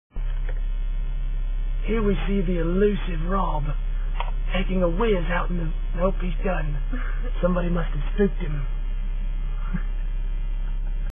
RobPee
We catch one of our good buddies relieving himself.